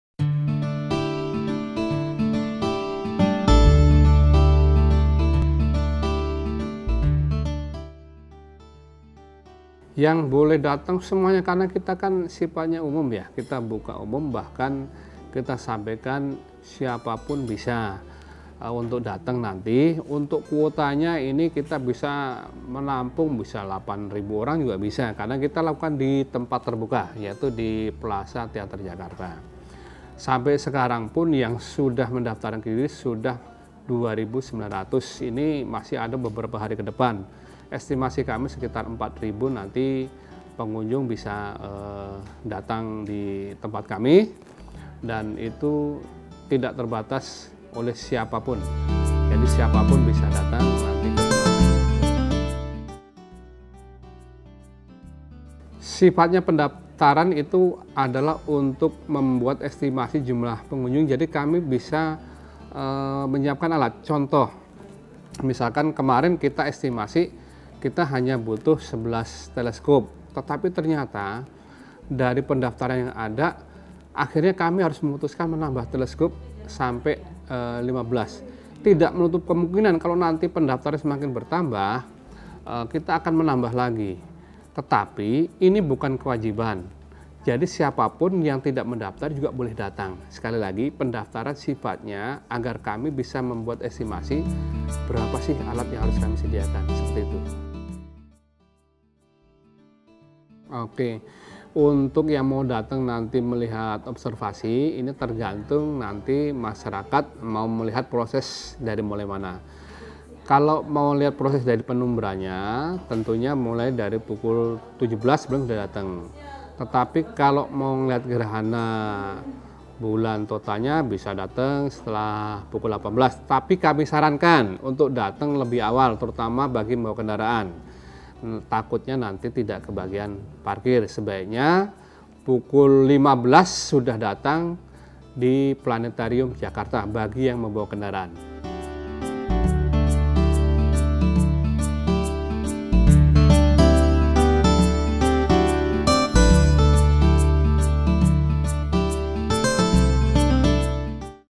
Planetarium Jakarta Akan Tampung Ribuan Pengamat Bulan Merah Darah | Bincang Kumparan
Eko Wahyu Wibowo, Kepala Satuan Pelaksana Teknis Pertunjukan dan Publikasi Unit Pengelola Pusat Kesenian Jakarta Taman Ismail Marzuki, menjelaskan bahwa menurut hasil estimasi yang Planetarium lakukan, ada ribuan orang yang akan datang untuk mengikuti acara 'nobar' (nonton bareng) Bulan merah darah nanti.

"Estimasi kami sekitar 4 ribu nanti pengunjung bisa datang," ujar Eko saat ditemui kumparan di kantornya.

Hasil estimasi tersebut didapatkan setelah Planetarium  membuka pendaftaran acara pengamatan bersama Bulan merah darah di situs resminya. Hingga Jumat, 26 Januari kemarin, jumlah pendaftar telah mencapai 2.900 orang.

Membludaknya jumlah pendaftar ini membuat Planetarium memutuskan untuk menambah jumlah teleskop yang akan digunakan pada 31 Januari malam nanti.

“Kemarin kita estimasi, kita hanya butuh 11 teleskop. Tetapi ternyata dari pendaftar yang ada, akhirnya kami harus memutuskan menambah teleskop sampai 15,” tutur Eko.

Meski jumlah pendaftar sudah banyak, pihak Planetarium akan tetap membuka pendaftaran ini bagi siapa pun. Bahkan, yang belum mendaftar pun tetap bisa langsung datang ke acara pengamatan bersama yang berlokasi di Plaza Teater Jakarta itu.

"Sifatnya pendaftaran itu adalah untuk membuat estimasi jumlah pengunjung. Jadi kami bisa menyiapkan alat," jelas Eko.

Pendaftaran bukanlah syarat atau kewajiban agar bisa mengikuti acara tersebut. “Jadi siapa pun yang tidak mendaftar juga boleh datang," tambahnya.

Acara pengamatan bersama di Plaza Teater Jakarta, yang menurut Eko bisa menampung hingga 8.000 orang, ini bersifat gratis. Pengunjung tidak akan dikenakan biaya apa pun.